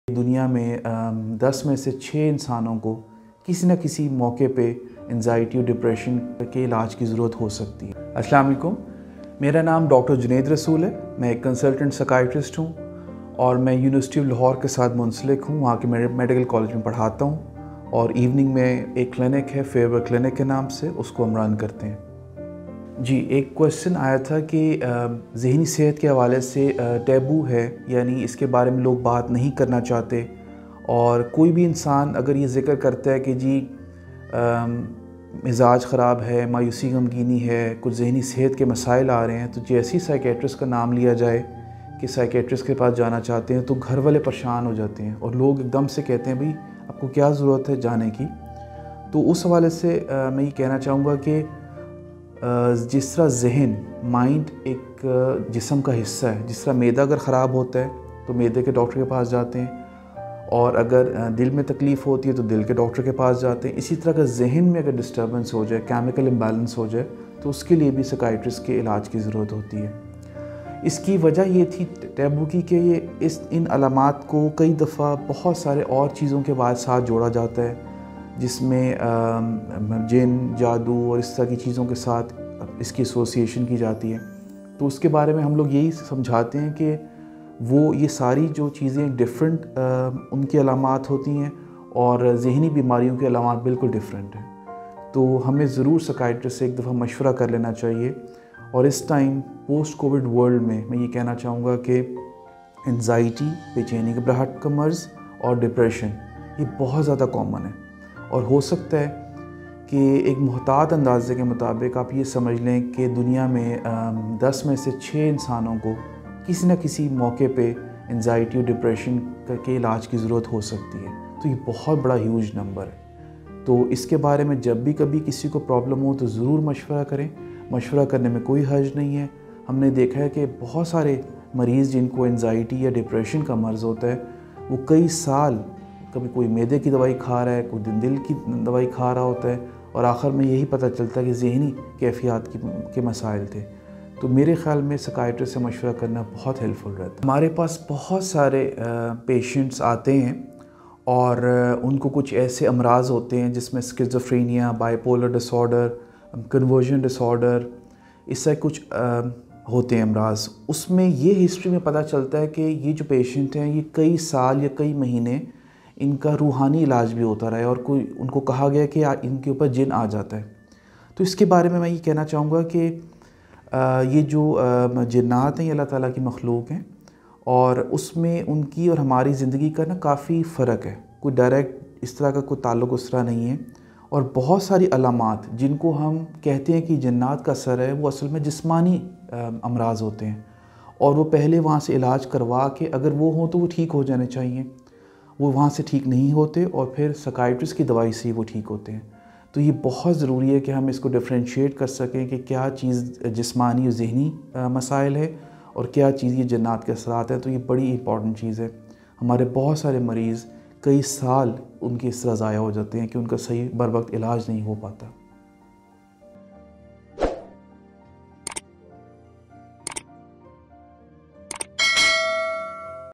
Depression, Baichaini, Mental Illness | Depression Anxiety Relief & Treatment | Dr. Junaid Rasool
Welcome to our latest video on anxiety and depression! In this informative video, we are joined by Dr. Junaid Rasool, an experienced Psychiatrist who will discuss the symptoms and treatment options available for anxiety and depression.

Anxiety and depression are two of the most common mental health disorders, affecting millions of people worldwide. In this video, we'll provide you with a better understanding of these conditions and how to manage them.

In this video, Dr. Junaid Rasool, top psychiatrist in Lahore will answer the most commonly asked questions like depression ka ilaj kya hai? Depression ki alamat aur depression se bahar kaise aaye?

Dr. Junaid Rasool will begin by defining anxiety relief and depression and discussing the symptoms associated with each disorder.

Dr. Junaid Rasool, best psychiatrist in Pakistan will also explain the importance of understanding that mental illness is a medical condition that is caused by a variety of factors, including genetics, environment, and life experiences. While cultural beliefs and practices can play a role in how mental health is perceived and treated, it is important to recognize the importance of seeking professional medical help from trained mental health experts.

If you or someone you know is struggling with anxiety, depression, OCD, bipolar, or any other mental health condition, this video is a must-watch. With Dr. Junaid Rasool's extensive knowledge and expertise in the field of psychiatry, you'll gain valuable insights into these conditions and how to seek the right help.